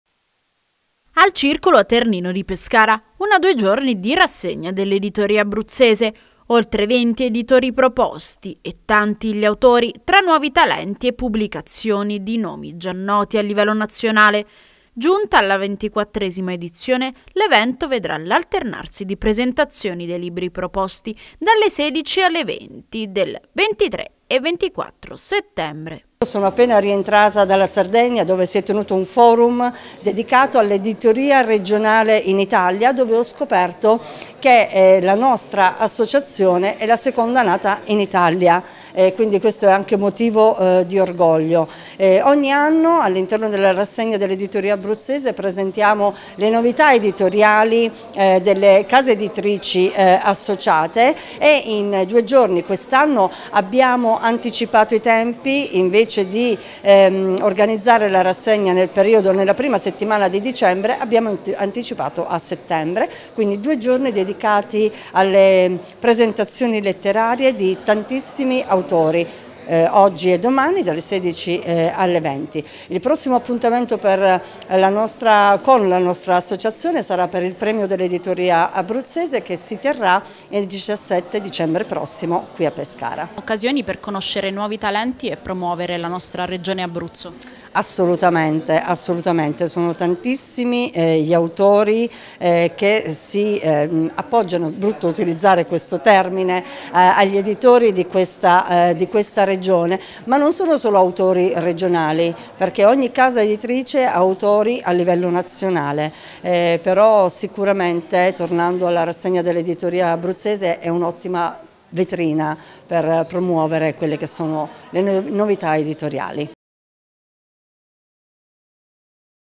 Rassegna dell'editoria regionali
Una due giorni di rassegna dell'editoria abruzzese al circolo aternino a Pescara tra nuove proposte e pubblicazioni di autori nazionali.